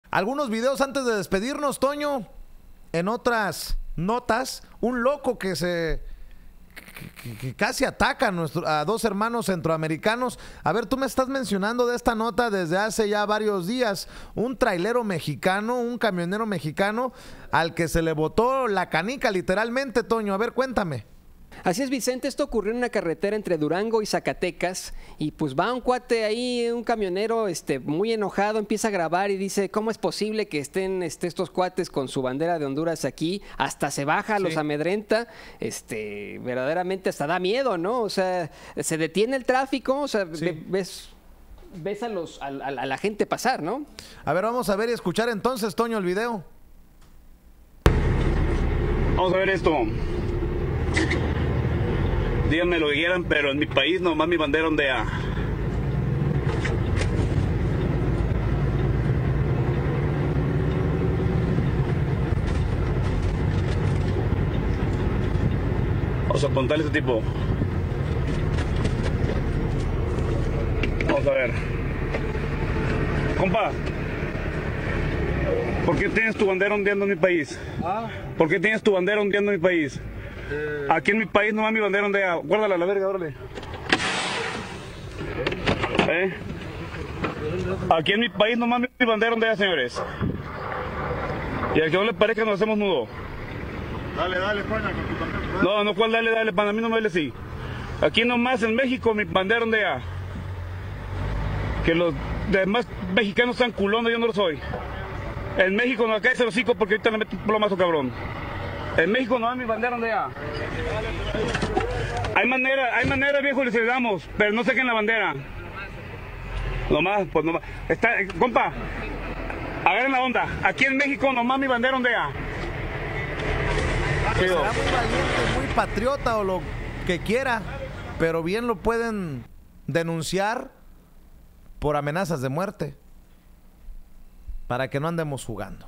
SE VUELVE LOCO CAMIONERO: EN MÉXICO NOMÁS MI BANDERA ONDEA - MEXICANO AMENAZA DE MUERTE A HONDUREÑOS
Un camionero amedrentó y amenazó de muerte a dos hondureños por traer sus banderas en una carretera entre Durango y Zacatecas